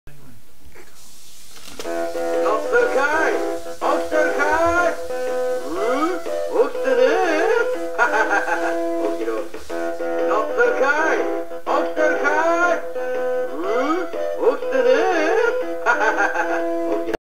Chicken Clock
This is the Chicken Alarm clock, mainly found on EB*Y, but this is the JAPANESE version!  Very rare!